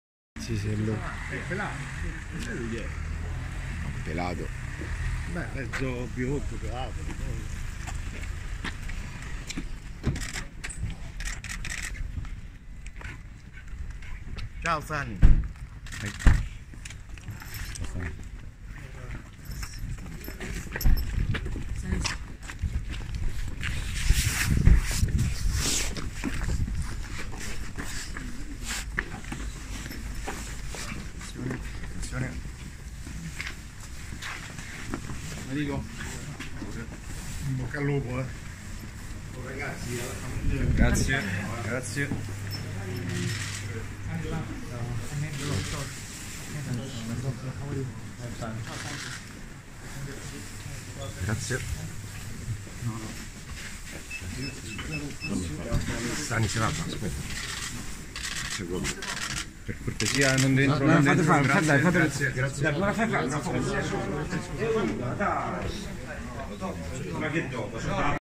IAMNAPLES.IT - Lobotka a Villa Stuart per le visite mediche col Napoli
Stanislav Lobotka del Celta Vigo sarà il prossimo acquisto del Napoli, stamane è arrivato a Villa Stuart per le visite mediche